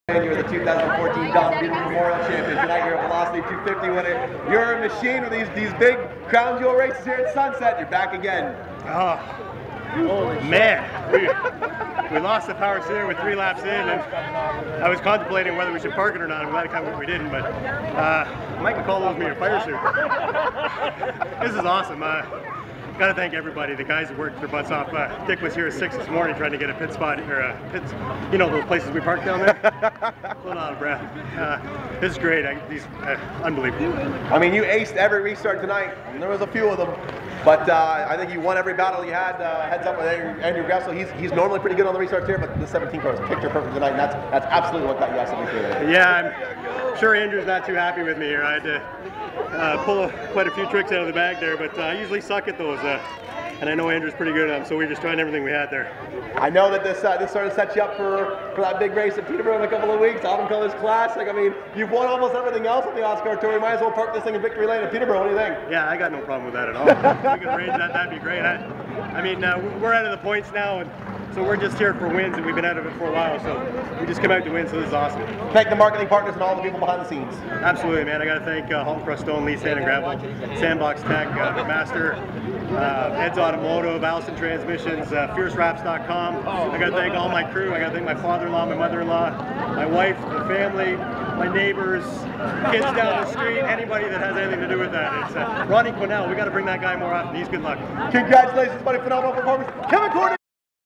2014-09-28 Sunset Speedway VELOCITY 250 Kevin Cornelius OSCAAR SLM Victory Lane Interview
Former Sunset Speedway Ltd. Late Model champion Kevin Cornelius earned his 2nd Crown Jewel victory of the season, taking the OSCAAR Super Late Model Tour portion of the Velocity 250 on September 28th.